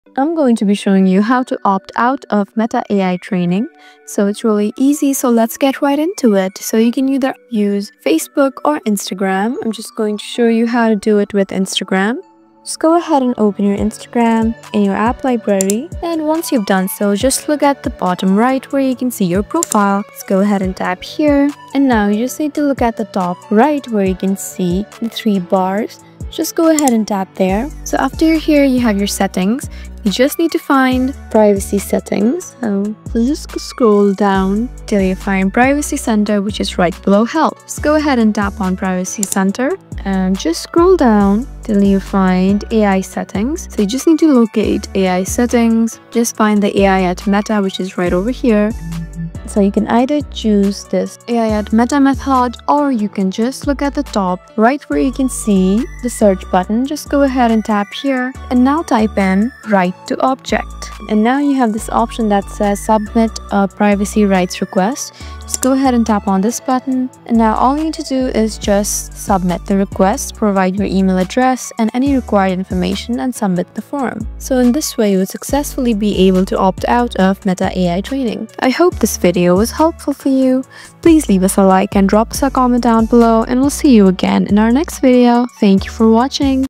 How To Opt Out Of Meta AI Training (Full Tutorial)
In this video I'll be showing you how to opt out of Meta AI training. So watch the video till the video till the end so that you won't miss any steps on how to opt out of Meta AI training.